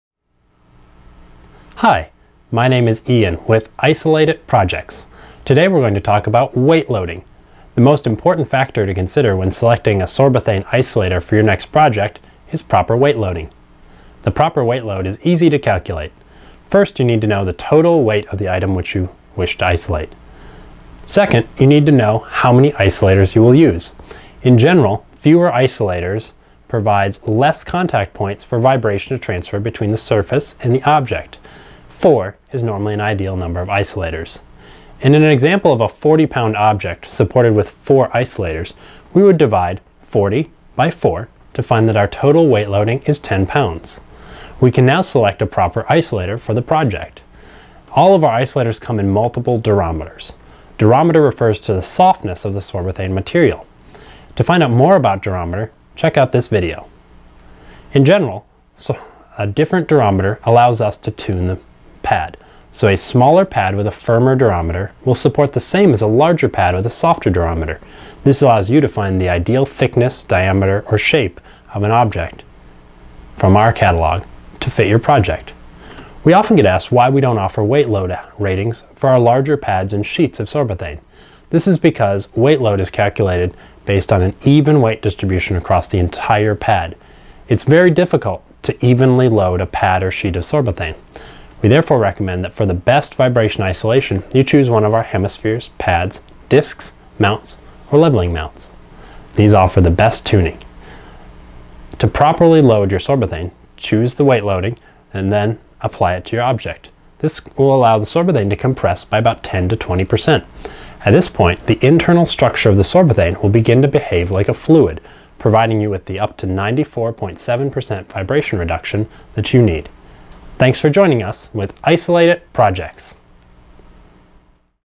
Isolate IT Projects - Calculating Weigh Loads
This video explains how to calculate the weight load for your project using Isolate IT Sorbothane vibration isolators. To maximize the vibration isolation properties of Sorbothane and get closest to it's full capacity to absorb and isolate up to 94.7% of vibration weight loading is key. Calculating weight load is as simple as dividing the total weight of the item to be isolated by the number of pads/isolators. In most cases 4 is an ideal number of isolators. Ex: a 40lb item isolated by 4 pads has a weight load per pad of 10lbs. 40 lbs ÷ 4 = 10lbs. Find out more about weight loading and Isolate IT Sorbothane